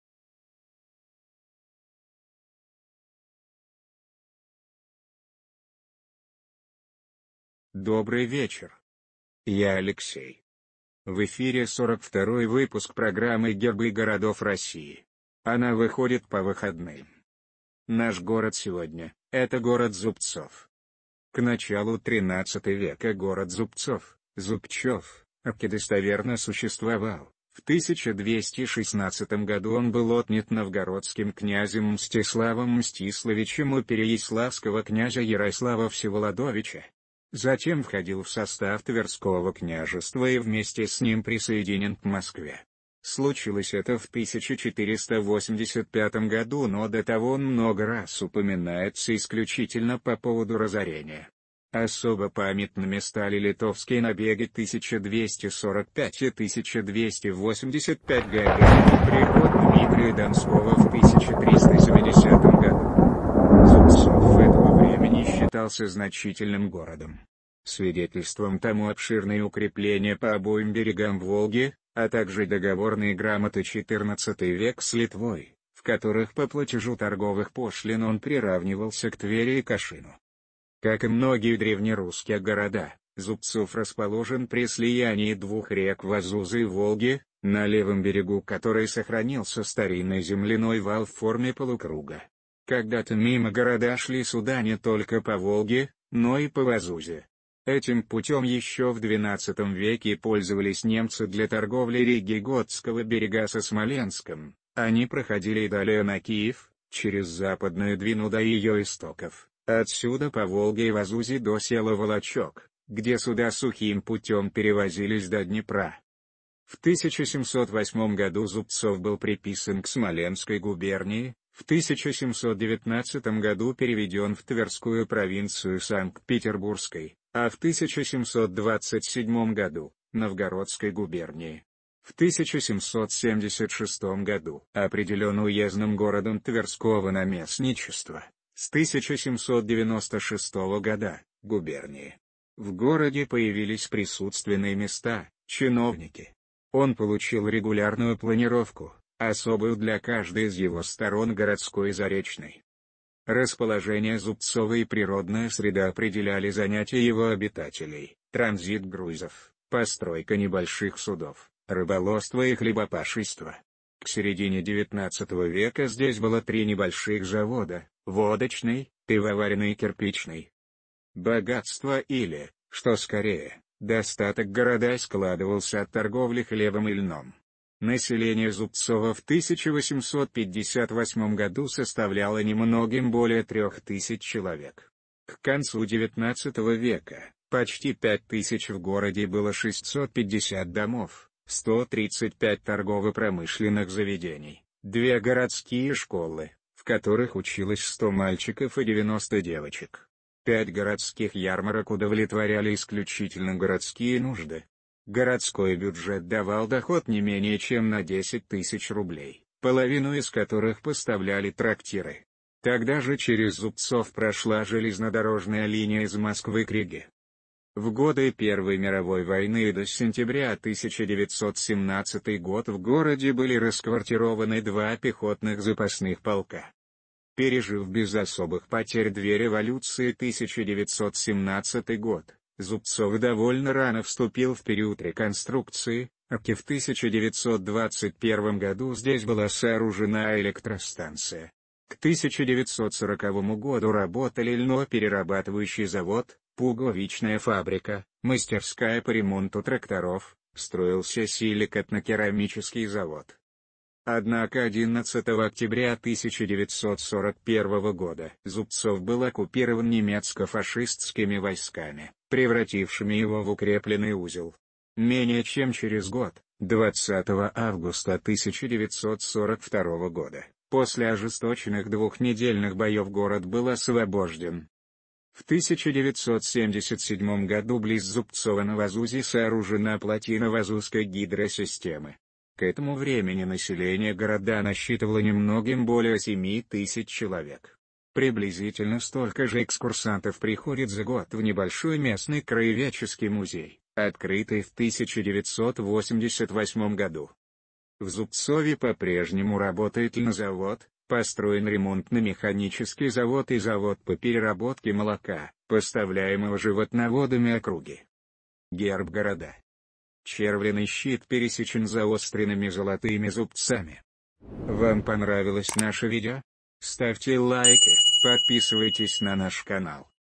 Гербы городов России. ЗУБЦОВ. Выпуск № 42. 18 декабря.
Помощь каналу Гербы городов России. ЗУБЦОВ. Выпуск № 42. 18 декабря.
К началу XIII в. город Зубцов (Зубьчев) рке достоверно существовал: в 1216 г. он был отнят новгородским князем Мстиславом Мстиславичем у переяславского князя Ярослава Всеволодовича. Затем входил в состав Тверского княжества и вместе с ним присоединен к Москве. Случилось это в 1485 г. Но до того он много раз упоминается исключительно по поводу разорения. Особо памятными стали литовские набеги 1245 и 1285 гг., приход Дмитрия Донского в 1370 г.
Зубцов этого времени считался значительным городом. Свидетельством тому — обширные укрепления по обоим берегам Волги, а также договорные грамоты XIV в. с Литвой, в которых по платежу торговых пошлин он приравнивался к Твери и Кашину.
Как и многие древнерусские города, Зубцов расположен при слиянии двух рек — Вазузы и Волги, на левом берегу которой сохранился старинный земляной вал в форме полукруга. Когда-то мимо города шли суда не только по Волге, но и по Вазузе (гжатский караван). Этим путем еще в XII в. пользовались немцы для торговли Риги и Готского берега со Смоленском, они проходили и далее на Киев: через Западную Двину до ее истоков, а отсюда по Волге и Вазузе до села Волочек, где суда сухим путем перевозились до Днепра.
В 1708 г. Зубцов был приписан к Смоленской губернии, в 1719 г. переведен в Тверскую провинцию Санкт-Петербургской, а в 1727 г.— Новгородской губернии. В 1776 г. определен уездным городом Тверского наместничества (с 1796 г.— губернии). В городе появились присутственные места, чиновники. Он получил регулярную планировку, особую для каждой из его сторон — городской и заречной.
Расположение Зубцова и природная среда определяли занятия его обитателей: транзит грузов, постройка небольших судов, рыболовство и хлебопашество. К середине XIX в. здесь было три небольших завода: водочный, пивоваренный и кирпичный.
Богатство или, что скорее, достаток города складывался от торговли хлебом и льном. Население Зубцова в 1858 г. составляло немногим более 3000 человек. К концу XIX в.— почти 5 тыс. В городе было 650 домов, 135 торгово-промышленных заведений, две городские школы, в которых училось 100 мальчиков и 90 девочек. 5 городских ярмарок удовлетворяли исключительно городские нужды. Городской бюджет давал доход не менее чем на 10 тыс. рублей, половину из которых поставляли трактиры. Тогда же через Зубцов прошла железнодорожная линия из Москвы к Риге.
В годы первой мировой войны до сентября 1917 г. в городе были расквартированы два пехотных запасных полка.
Пережив без особых потерь две революции 1917 г., Зубцов довольно рано вступил в период реконструкции: рке в 1921 г. здесь была сооружена электростанция. К 1940 г. работали льноперерабатывающий завод, пуговичная фабрика, мастерская по ремонту тракторов, строился сили-катно-керамический завод.
Однако 11 октября 1941 г. Зубцов был оккупирован немецко-фашистскими войсками, превратившими его в укрепленный узел. Менее чем через год, 20 августа 1942 г., после ожесточенных двухнедельных боев город был освобожден.
В 1977 г. близ Зубцова на Вазузе сооружена плотина Вазузской гидросистемы. К этому времени население города насчитывало немногим более 7 тыс. человек. Приблизительно столько же экскурсантов приходят за год в небольшой местный краеведческий музей, открытый в 1988 г.
В Зубцове по-прежнему работает льнозавод, построен ремонтно-механический завод и завод по переработке молока, поставляемого животноводами округи.
Герб города
Червленый щит пересечен заостренными золотыми зубцами.